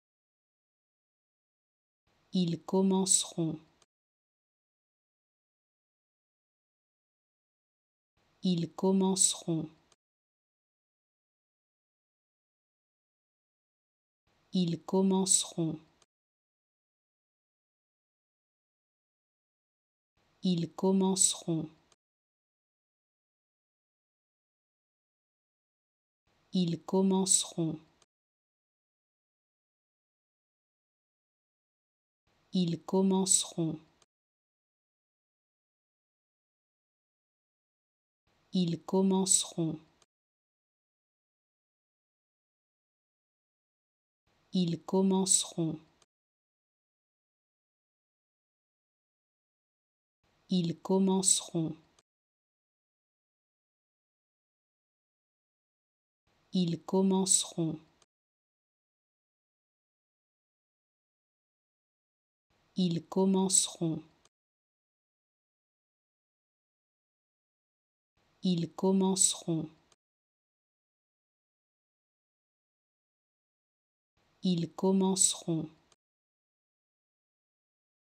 How to Pronounce ''Ils commenceront'' (They will start) Correctly in French
Learn how to say and properly pronounce ''Ils commenceront'' in French with this free pronunciation tutorial.